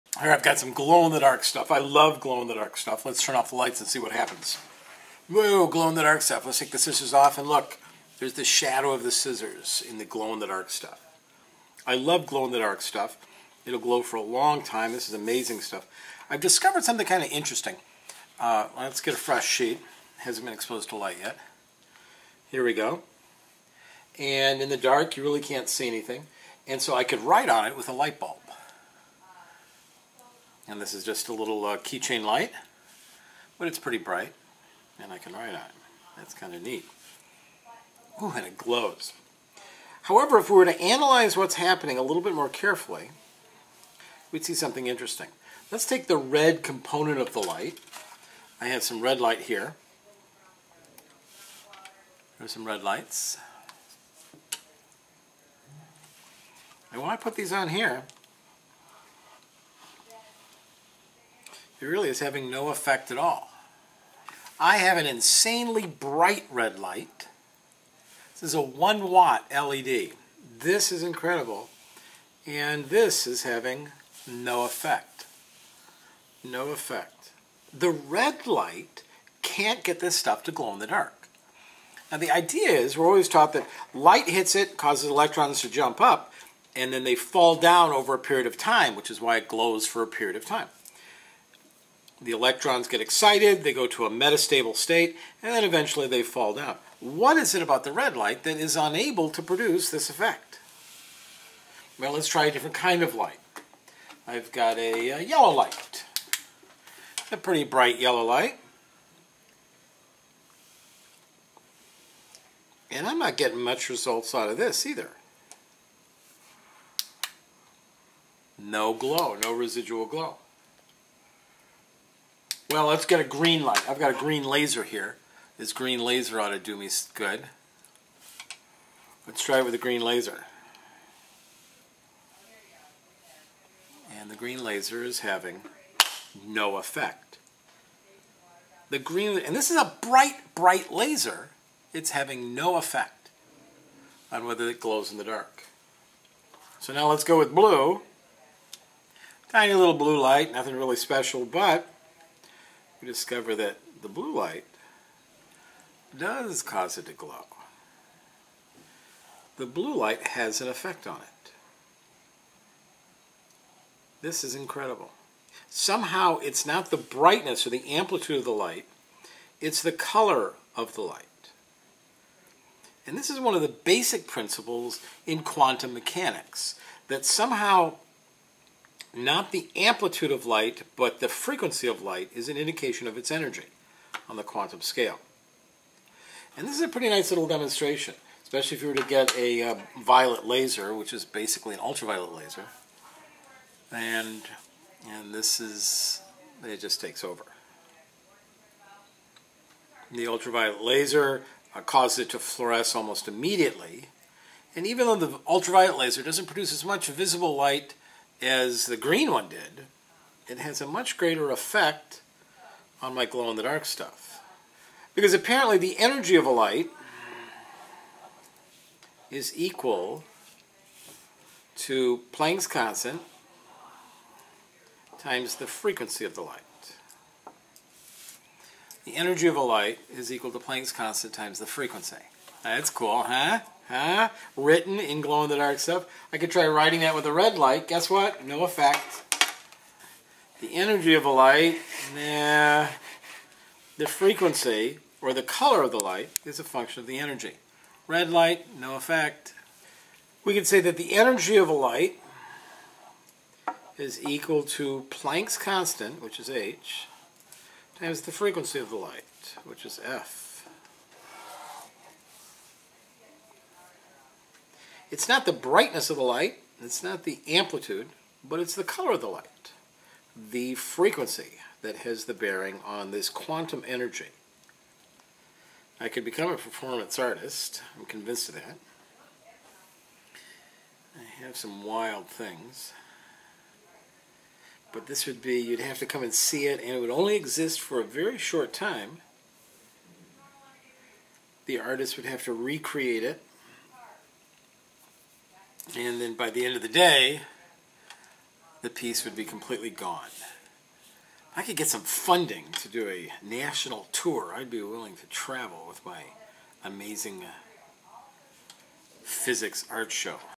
Quantum Physics illuminated (4 McAyLa)
Using simple tools it is easy to demonstrate E=hf